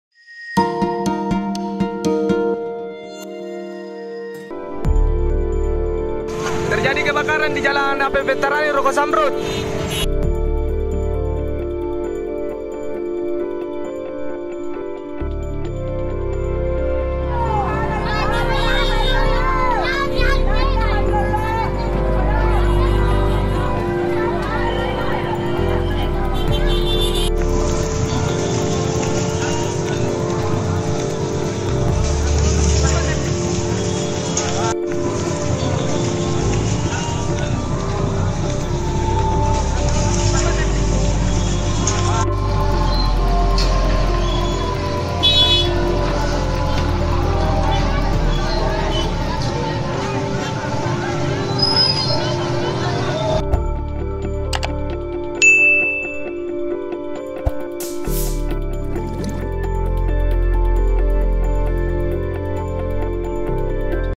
Komplek Ruko New Zamrud Pettarani Makassar Terbakar
SULSELSATU.com - Kebakaran terjadi di kompleks ruko New Zamrud, Jalan Andi Pangerang Pettarani, Makassar, Sabtu (23/11/2019) malam.

Kebakaran tersebut terjadi yang terjadi pada pukul 20.15 Wita.

Sejumlah ruko dilalap api.

Beruntungnya tidak ada korban jiwa dalam kebakaran ini.

Belum diketahui penyebab kebakaran ini.